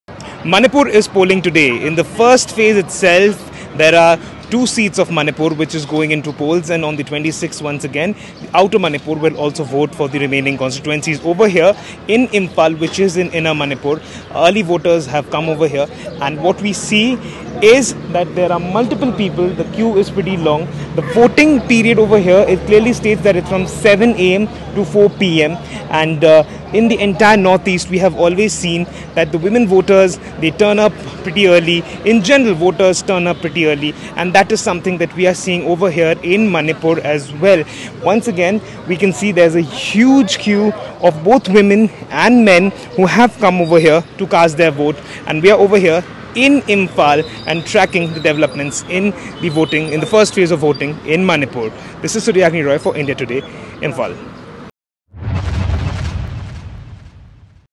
Lok Sabha Election 2024: First Phase of Voting in Manipur | India Today
In the first phase of Manipur's polling today, two constituencies are voting. On the 26th, outer Manipur will vote for the remaining constituencies. Early voters, including a large turnout of women, are seen in Impal, inner Manipur. The voting period is from 7 a.m to 4 p.m. The queue of voters is substantial, with both women and men turning up early to cast their votes.